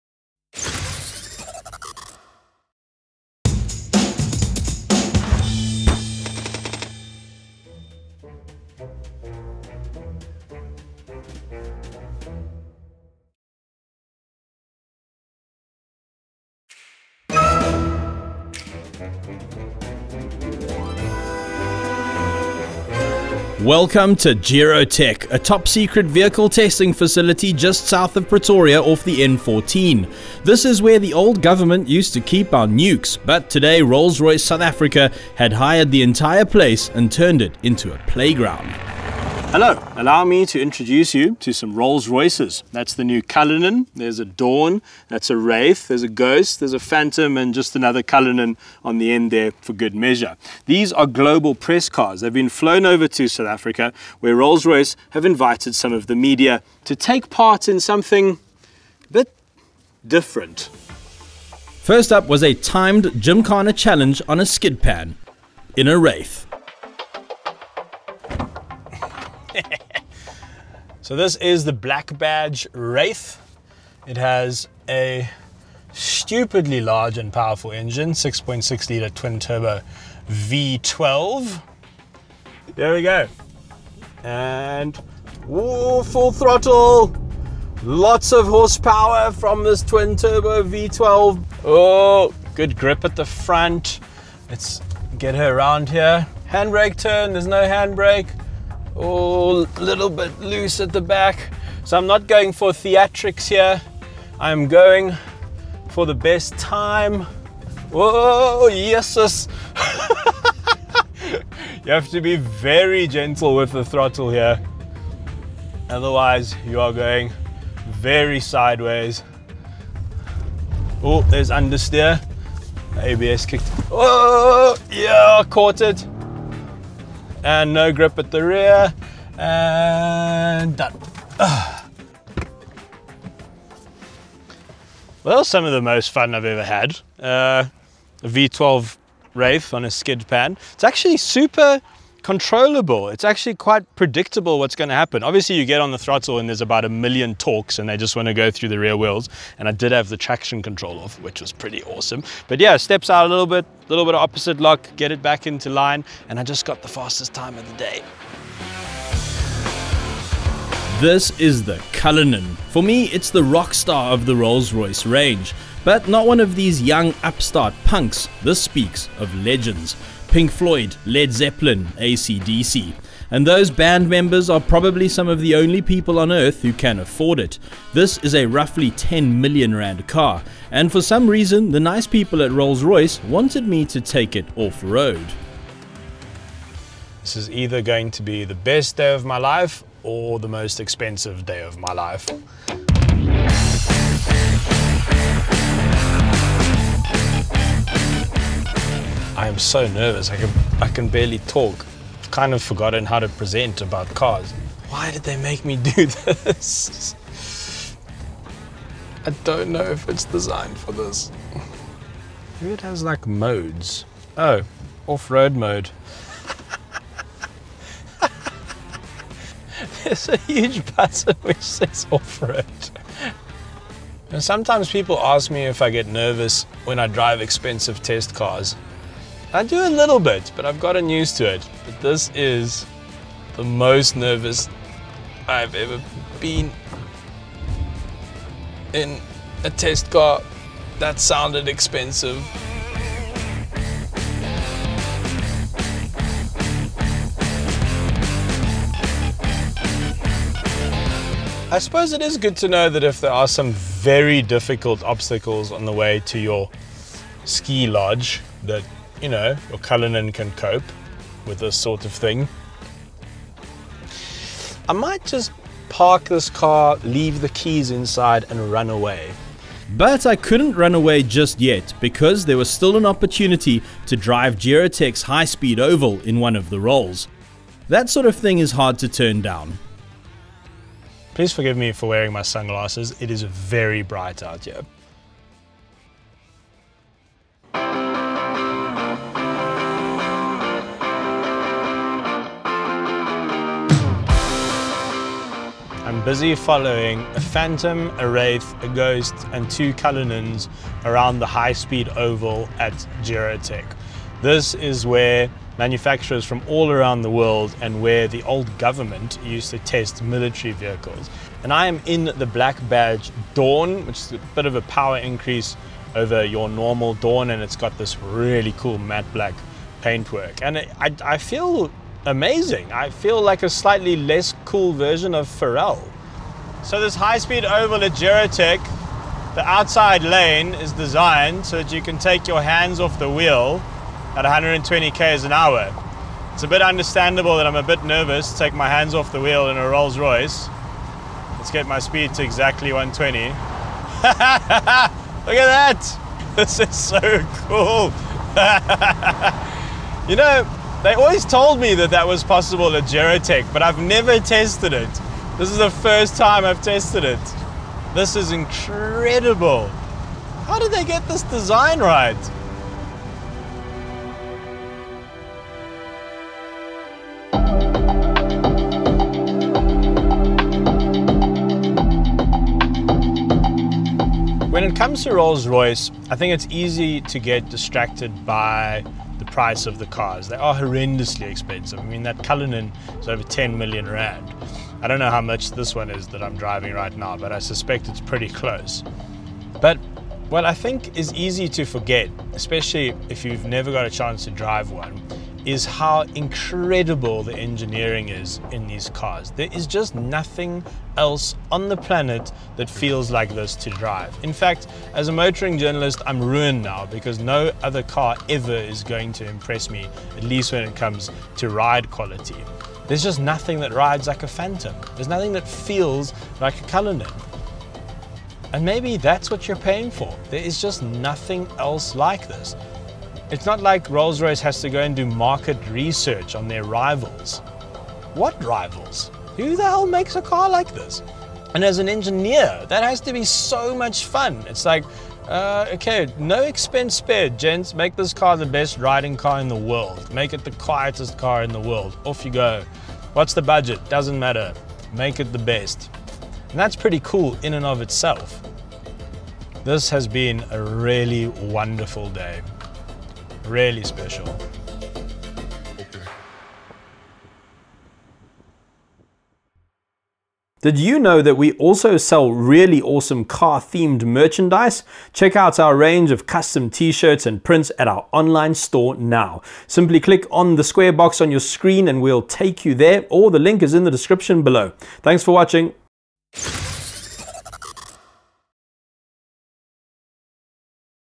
Doing Silly Things in Rolls Royces
Buy cool car stuff in our shop!

Rolls Royce global runs a fleet of press cars which are flown around the world for press and customer demonstrations. It's quite a sight; two Cullinans, a Phantom, a Dawn and a Black Badge Wraith coupe travel across the globe like a very comfortable, expensive circus.

Rolls Royce South Africa decided to make the most of the opportunity and invited local media to the incredible Gerotek testing facility to experience these vehicles in some rather unique ways...

Ciro De Siena was lucky enough to take part on behalf of Cars.co.za and in this video you'll see him drive a Wraith on a wet skidpan with the traction control turned off, take a R10million Cullinan SUV offroad and finally take a spin around Gerotek's high speed oval in a convertible Dawn.

Lucky sod.

0:00 Intro
0:39 Introduction to the Rolls Royces
1:02 Gymkhana challenge on a skid pad in a black badge wraith
1:15 Engine size and power
2:15 Commentary on car handling
2:40 Rolls-Royce Cullinan off-road
4:30 JIRA tech high speed oval in one of the Rolls-Royce
6:14 Pricing
6:30 Ride quality
7:40 Outro